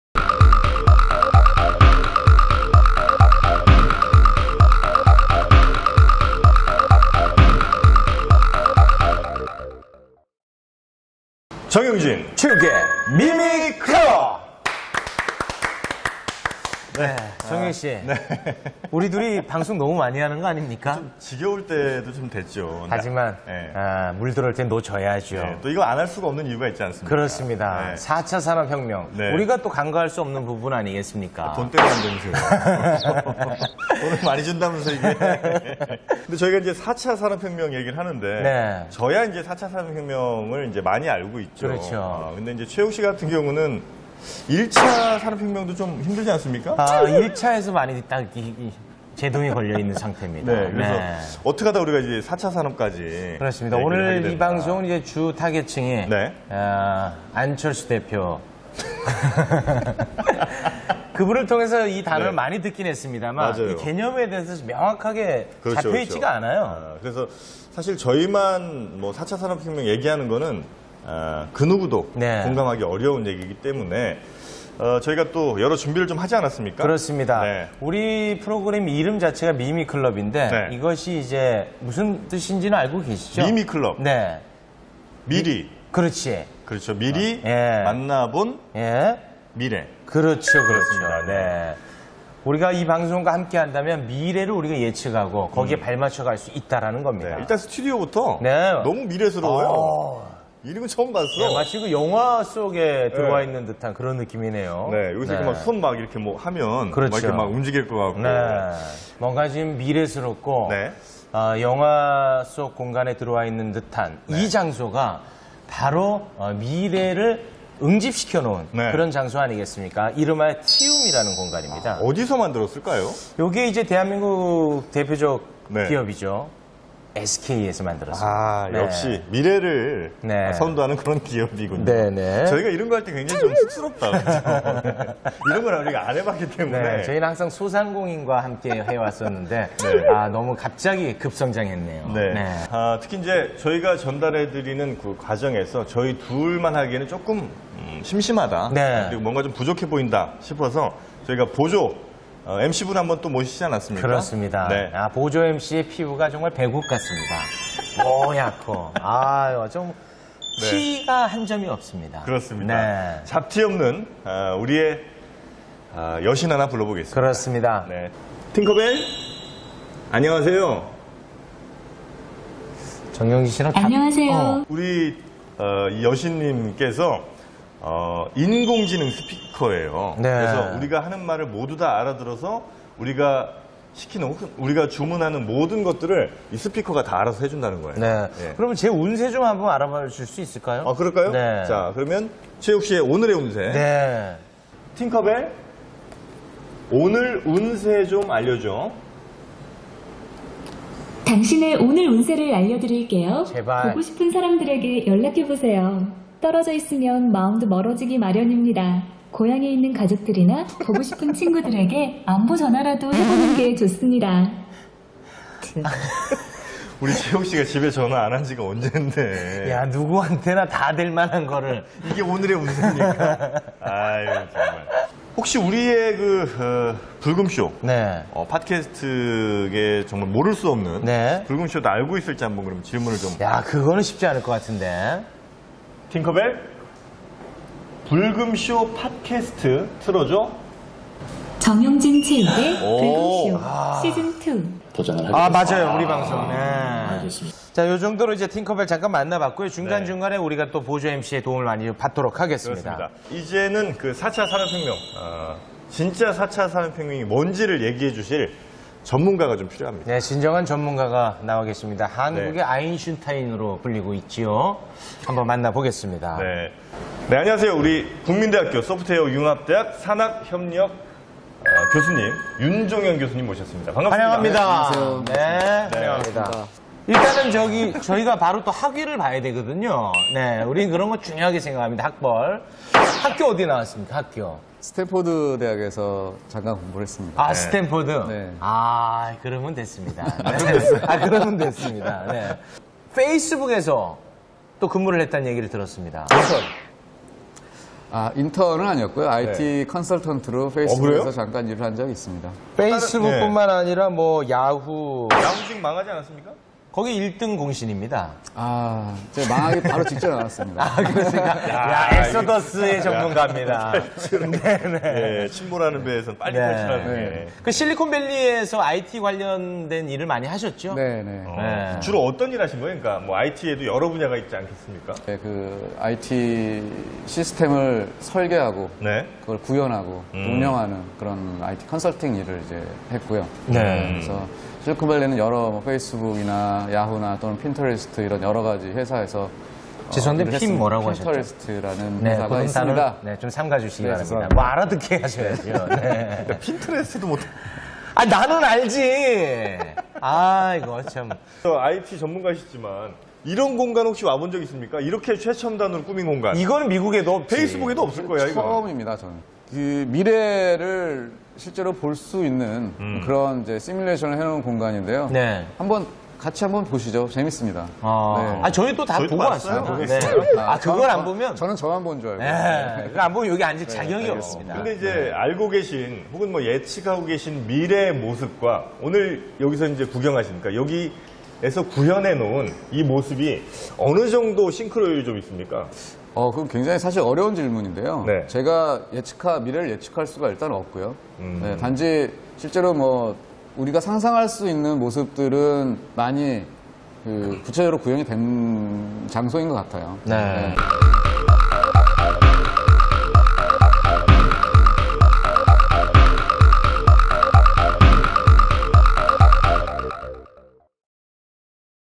[ZMTV]미미클럽 팅커벨과 떠나는 미래 여행!
SK텔레콤과 함께하는 4차 산업혁명!
유쾌한 두 남자 정영진 최욱과 함께 만나보세요!